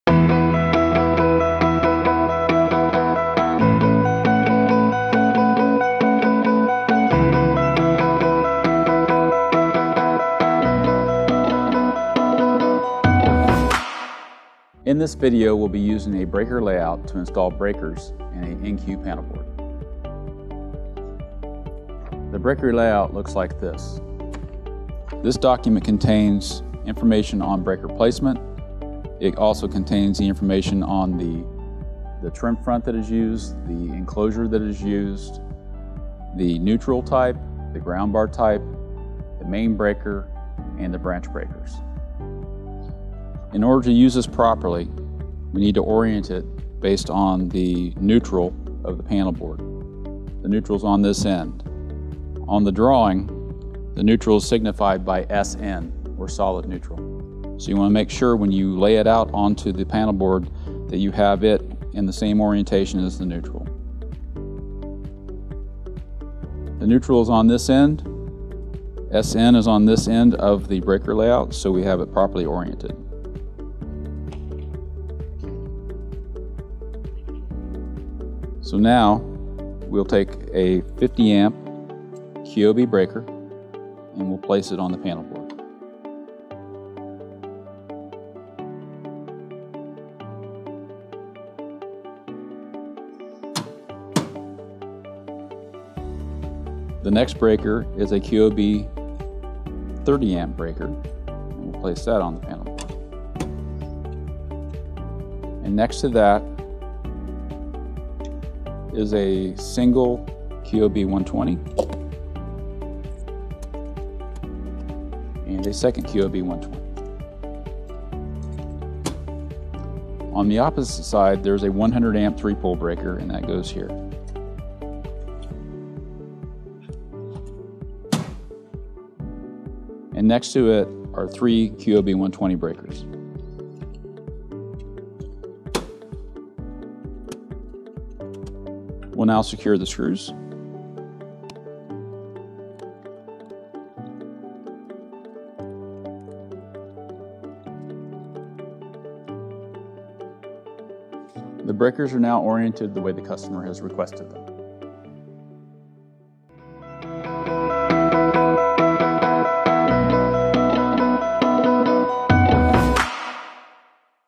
NQ NF RTI Intro (2 of 4) Panelboard Assembly: Single Line Overview
Part of the Square D™ NQ and NF RTI Series
This video demonstrates how to interpret a breaker layout. When building an RTI (Ready-to-Install) panelboard, a breaker layout, or single line overview, is used to install the breakers, per customer specification, into the panelboard.